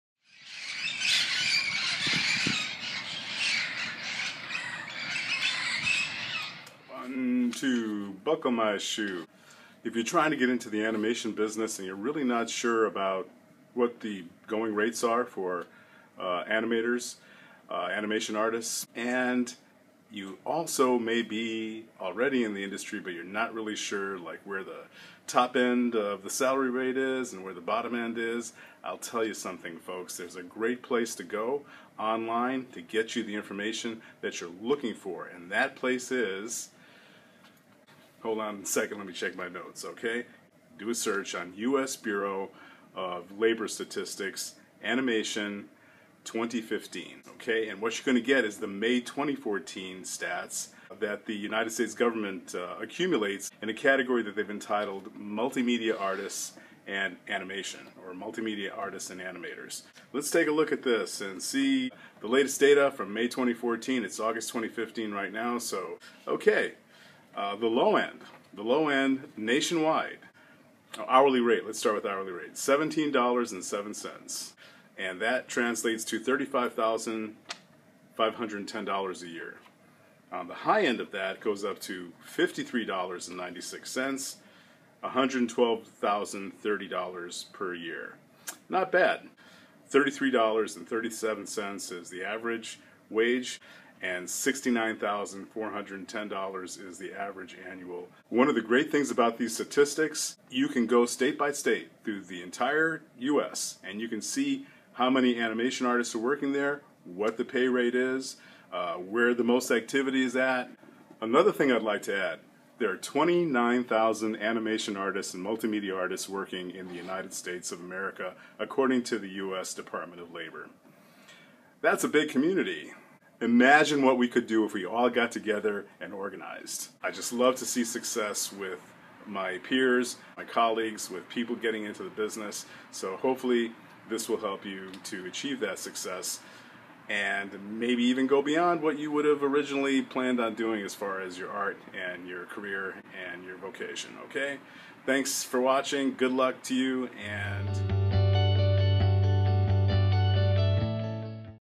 The Animation Academy - US Dept of Labor Animation Industry Stats 2014
The US Department of Labor publishes national employment statistics for animation and multimedia artists annually. The most current data is from May 2014. Follow the link below for comprehensive info concerning wages, categories, geographic distribution of workers and other data important to our understanding of the labor market in America.